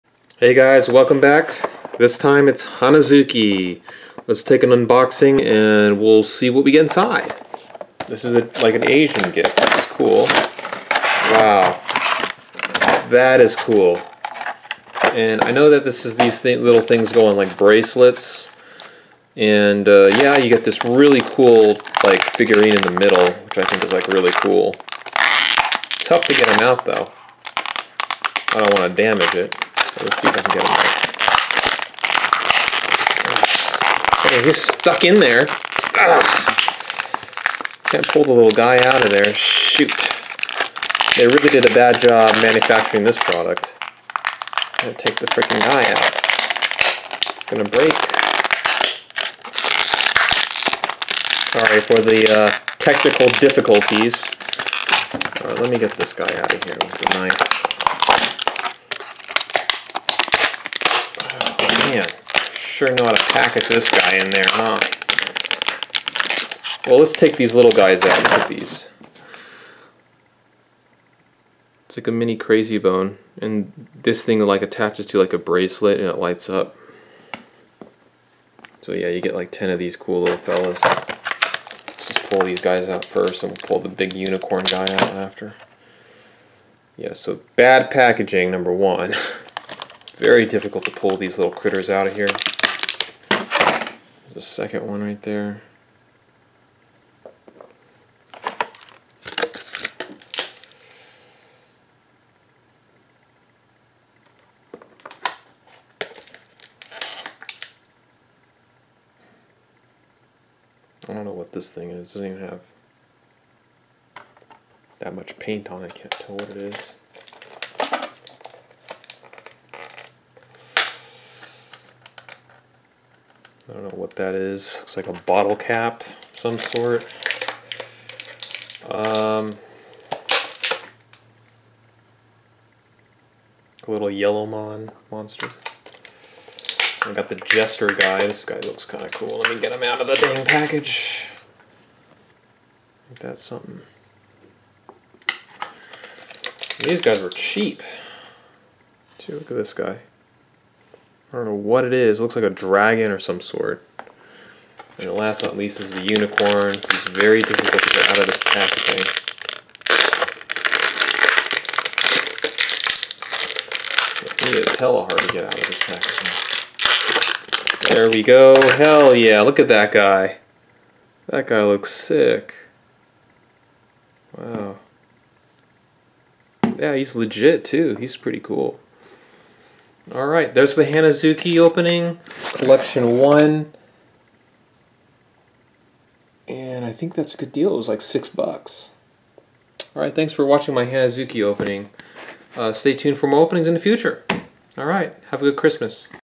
Hanazuki Unboxing
This is my unboxing of a great product - Hanazuki! Hope you enjoy and share. This product is hands down, bad ass.